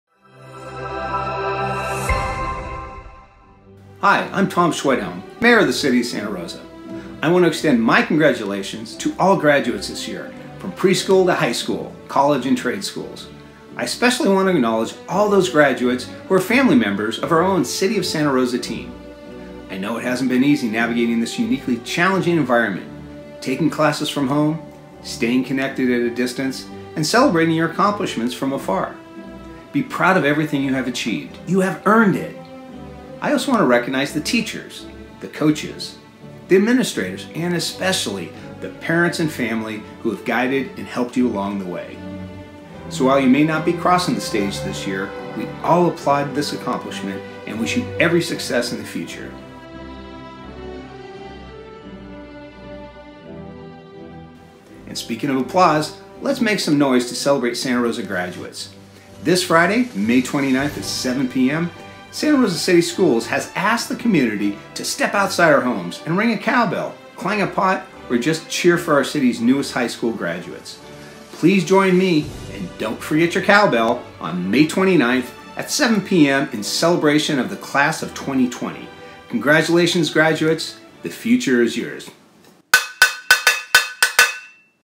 Congratulations, Class of 2020 from the City of Santa Rosa
The City of Santa Rosa congratulates all graduates this year, from pre-school to high school, college, and trade schools. We especially want to acknowledge all those graduates who are city employees and their family members too. While you may not be crossing the stage this year, we all applaud this accomplishment and wish you every success in the future. Congratulations!